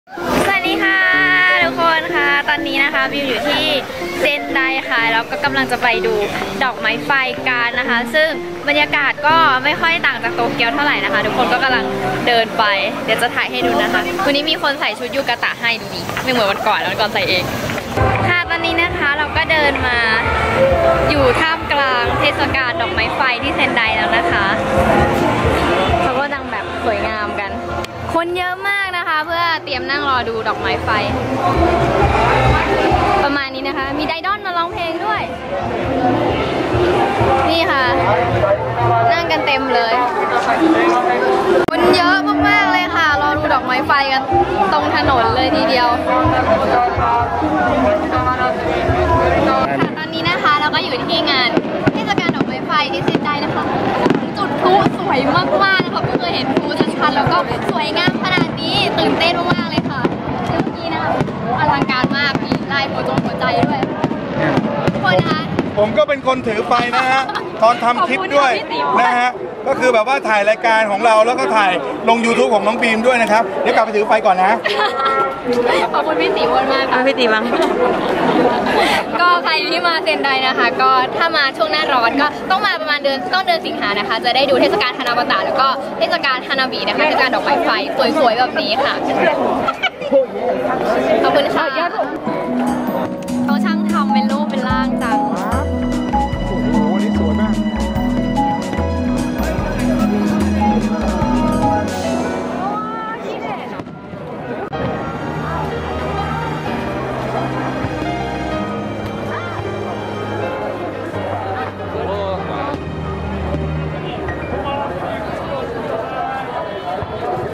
เที่ยวญี่ปุ่นกับ BeamSensei : เทศกาลดอกไม้ไฟ ณ เซนได
Hanabi ดอกไม้ไฟที่เซนไดค่ะ
มาดูดอกไม้ไฟ ที่เมืองเซนไดกันบ้างเร้ววววววววววว

O O !
ขอบคุณช่างไฟพิเศษด้วยค่ะ 555